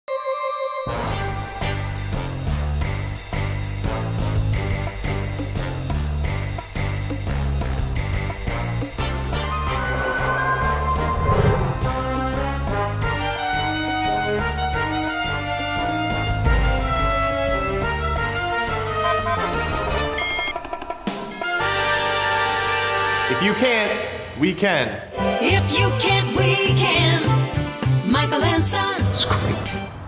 Tom Cruise in Mission Impossible Michael and Son commercial
Michael and Son partnered up with Tom Cruise for their latest TV commercial, based on the movie Mission Impossible.  This is the latest commercial in a series based on their tagline "If you can't, we can!" In this action-packed spot, the Michael & Son technician helps "Tom" successfully complete his mission. Fun fact: Although the resemblance is uncanny, this Cruise was a hired lookalike.